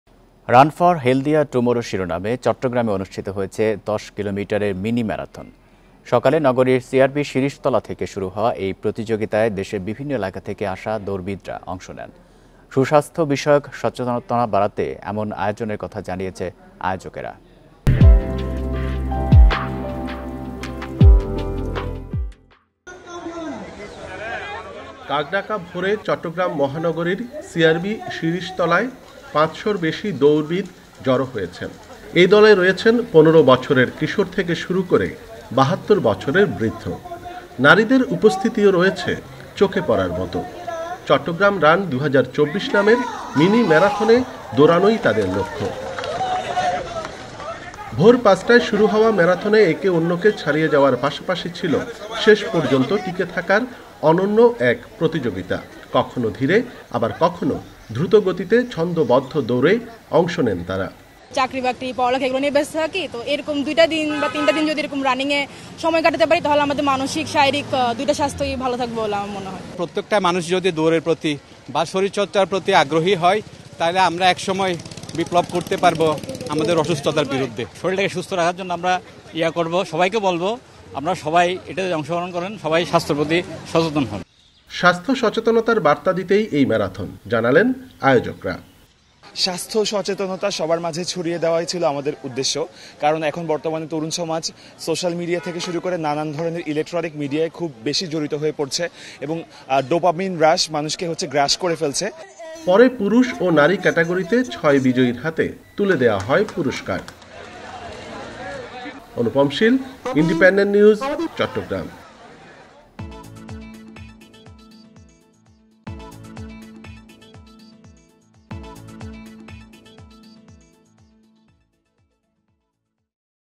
চট্টগ্রামে মিনি ম্যারাথন দৌড় প্রতিযোগিতা অনুষ্ঠিত | CTG News | Marathon | Independent TV
‘রান ফর হেলদিয়ার টুমোরো’ শিরোনামে চট্টগ্রামে অনুষ্ঠিত হয়েছে ১০ কিলোমিটারের মিনি ম্যারাথন। ((শুক্রবার)) সকালে নগরীর সিআরবি শিরীষতলা থেকে শুরু হওয়া এই প্রতিযোগিতায় দেশের বিভিন্ন এলাকা থেকে আসা দৌড়বিদরা অংশ নেন। সুস্বাস্থ্য বিষয়ক সচেতনতা বাড়াতে এমন আয়োজনের কথা জানিয়েছে আয়োজকরা।

A 10 km mini marathon was held in Chittagong under the title 'Run for Healthier Tomorrow'. Runners from different parts of the country took part in the competition which started from CRB Sirishtala in the morning ((Friday)). The organizers have said that the event will be organized to raise awareness about good health.